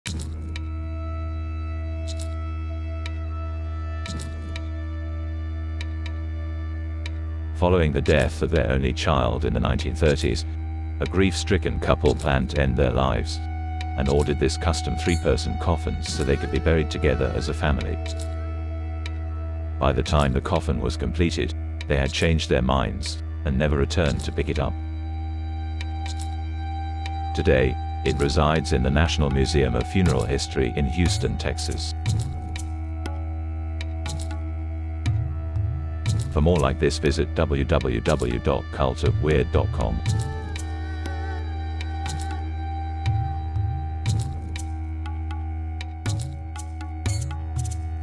3-Person Triple Casket - Why was it made?
The tragic story behind the "Casket for Three" on display in the National Museum of Funeral History in Houston, TX.